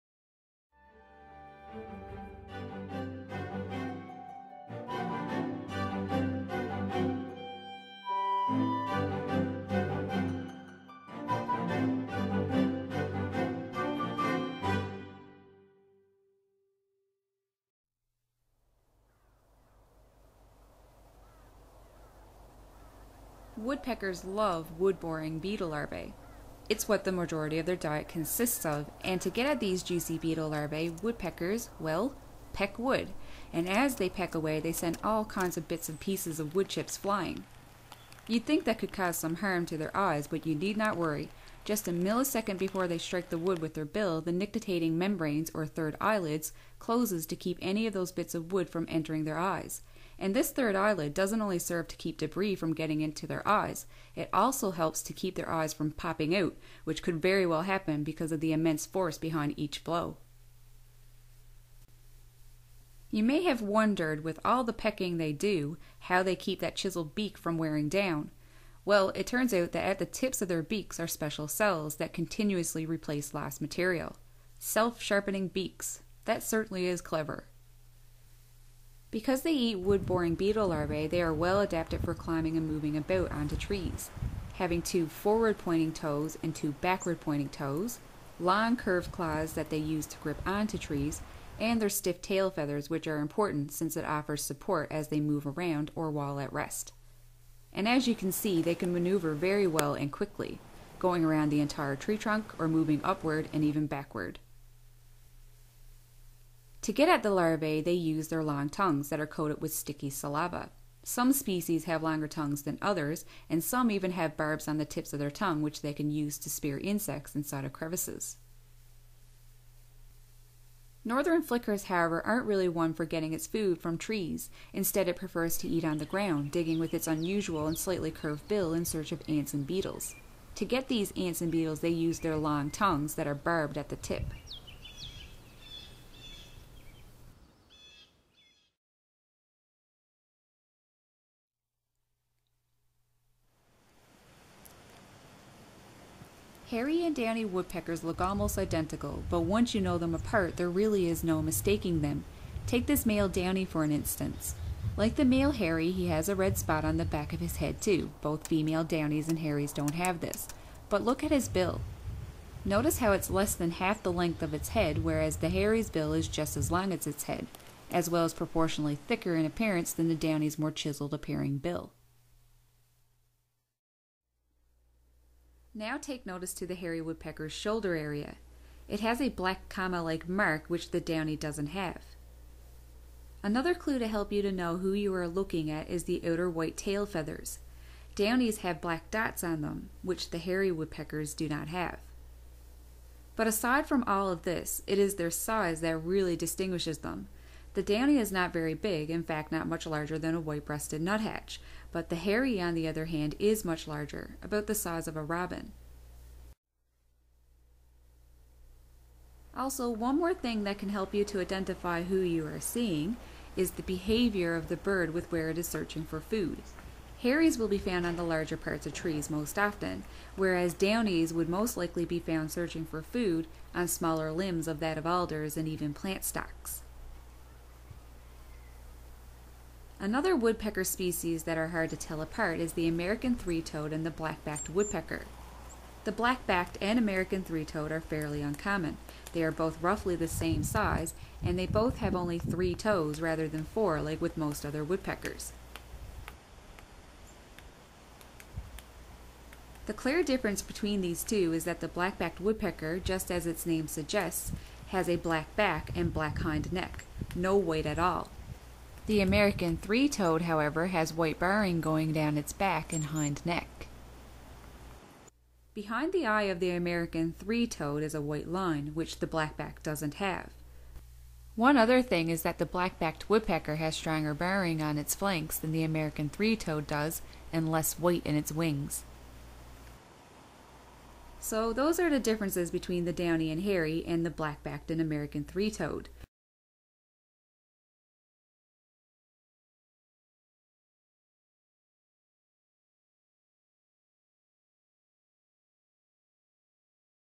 Woodpeckers Pt. 2 - How They Can Climb Trees - How To Tell Hairy and Downy Woodpeckers Apart
Woodpeckers are well adept at Climbing Trees as they search for insects, they have 4 toes; 2 backward pointing ones and two forward pointing ones with long curved claws which they use to grip onto trees, and their stiff tail feathers offers support as they move around or while at rest. Northern Flickers, however, look for their food(ants and beetles) on the ground digging with their unusual and slightly curved bill, and long tongue that are barbed at the end.  Some Woodpeckers, like the American Three-toed and Black-Backed woodpecker have only 3 toes; 2 forward pointing ones and just one backward pointing one.

Hairy and Downy Woodpeckers look very much a like but it is fairly easy to tell them apart given that the Downy isn't very big, not much larger than a white-breasted nuthatch, while the Hairy is much larger, the size of a American Robin. Another Woodpecker species that is quite similar is the Black-Backed woodpecker and the American Three-toed woodpecker. Unlike with the Downy and hairy, these two are roughly the same size and both have just 3 toes but the most obvious difference between them is that the American Three-toed has white barring going down it's back and hind neck whereas the Black-backed woodpecker back and hind neck is completely black, no white at all.